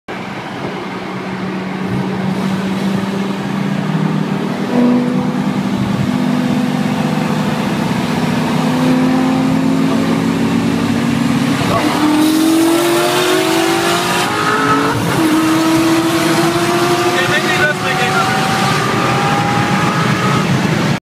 Procharged grandsport corvette vs built 3.0 7175 supra
Procharged grandsport corvette with cam headers meth vs 7175 supra with a built 3.0 e85 on 30psi round 2